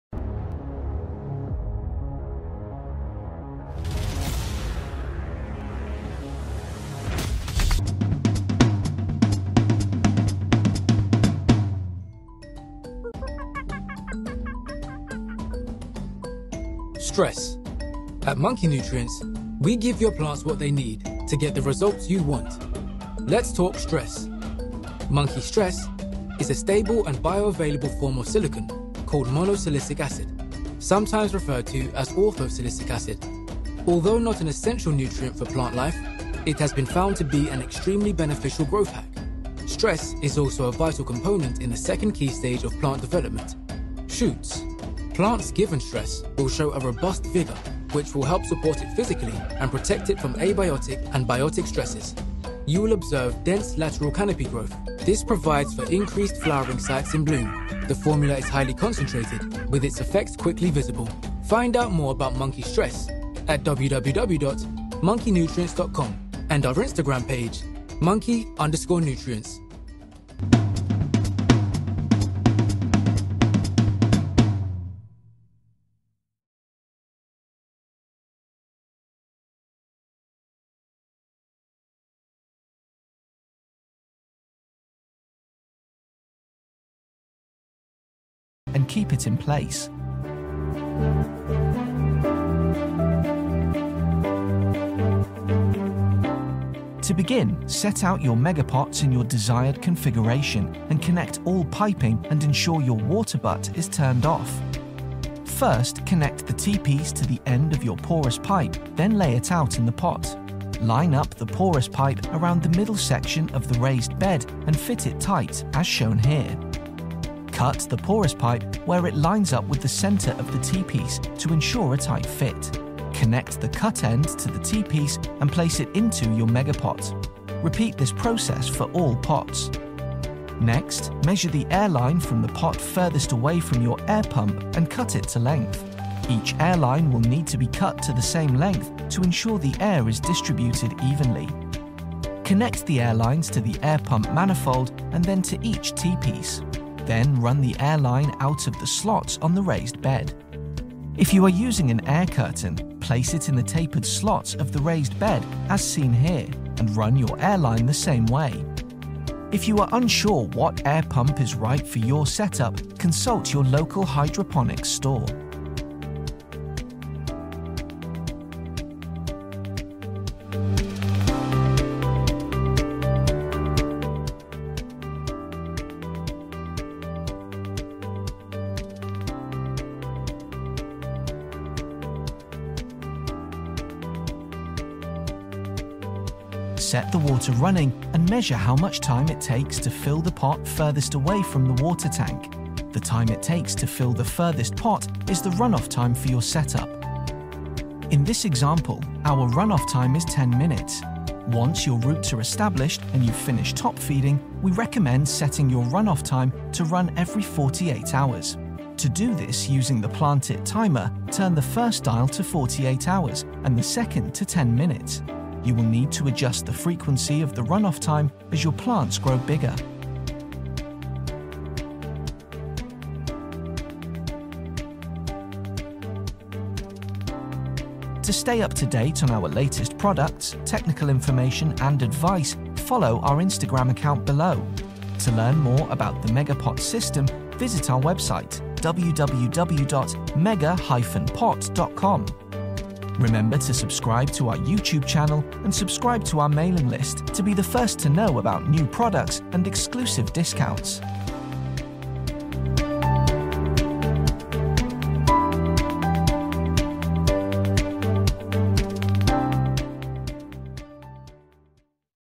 Monkey Nutrients Stress High Grade Hydroponics
Monkey Stress is a stable and bio-available form of silicon, called mono-silicic acid (also called Ortho-silicic acid). Although not an essential nutrient for plant life, it has been found to be an extremely beneficial growth hack. The formula is highly concentrated with it’s effects quickly visible. Plants given Stress will show a robust vigour, which will help support it physically and protect it from abiotic and biotic stresses.

Stress is also a vital component in the second key stage of plant development, Shoots. With stress you will observe dense lateral canopy growth. This provides for increased flowering sites in bloom.

Available Online with Next Day Delivery and In Store at High Grade Hydroponics.